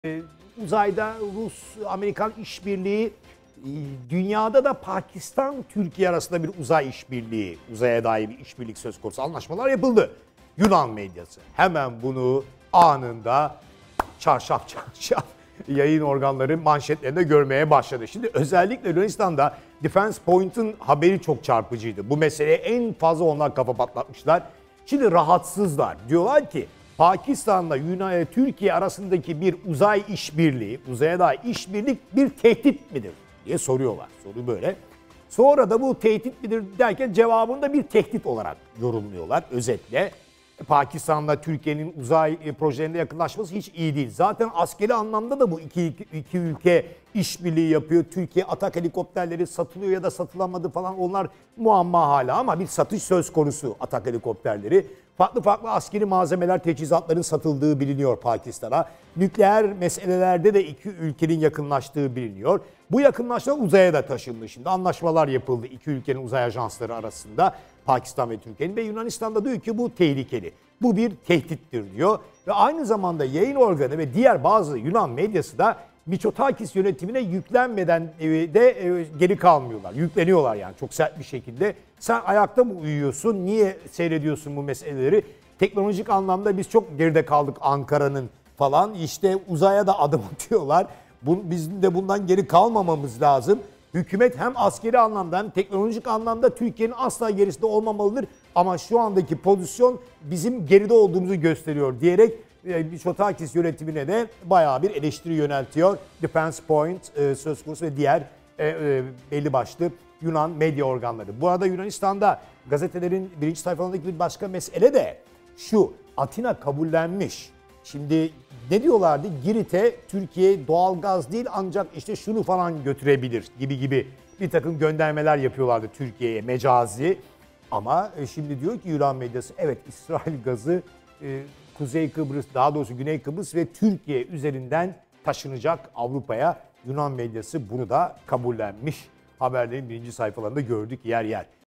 Türkiye - Pakistan Güç Birliği Yunanistan'ı Endişelendirdi - Tuna Öztunç ile Dünyada Bugün
Yunan medyası teknolojik açıdan Türkiye'den geri kalındığını itiraf ederek hükümeti geri kalmakla suçladı.